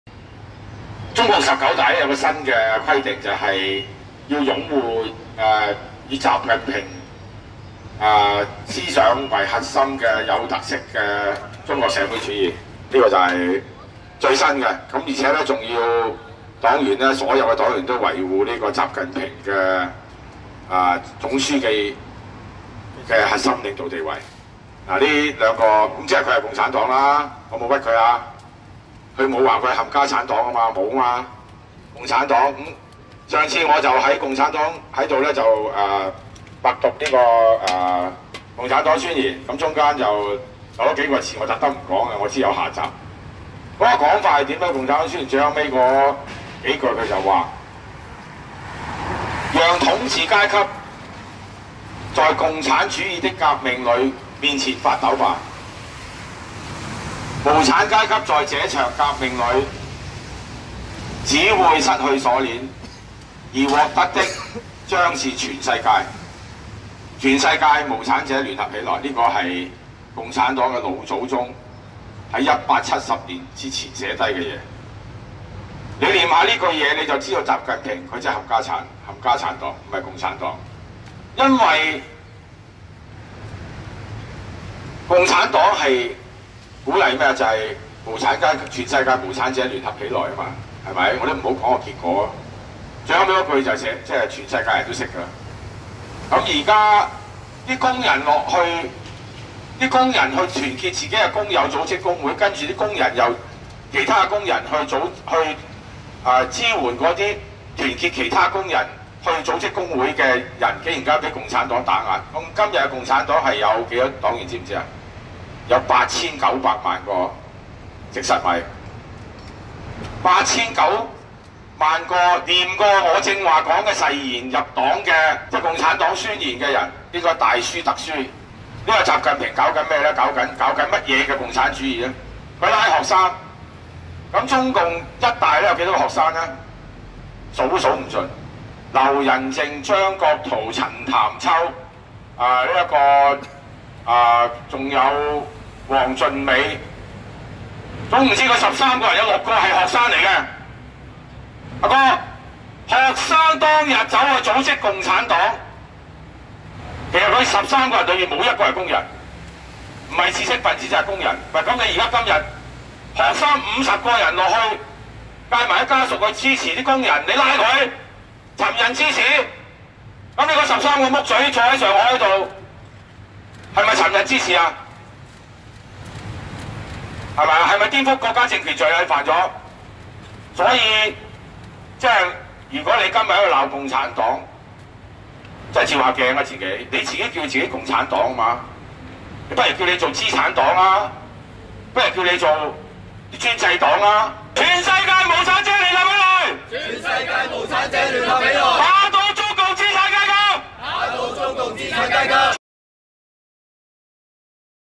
全世界無產者聯合起來聲援佳士工會
【佳士工人組織工會無罪】

共產黨宣言內有提及讓全世界無產者聯合起來，習近平如今統治的共產黨居然打壓無產者組織工會，那共產黨豈不是「冚家鏟」黨？！不如改名為資產黨吧！要知道中共創黨的十三人也大多是無產階級，中共如今竟拘捕50 多名聲援工運的學生實在太可恥！